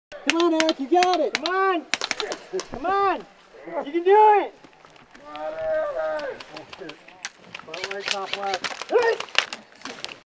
Come on Eric, I believe in You!
Our saviors bravely "encourage" the wounded EHouse to save himself in the final battle of Omega Productions Desolation 2 event.

Tik my Toks! - @ninjaofcameras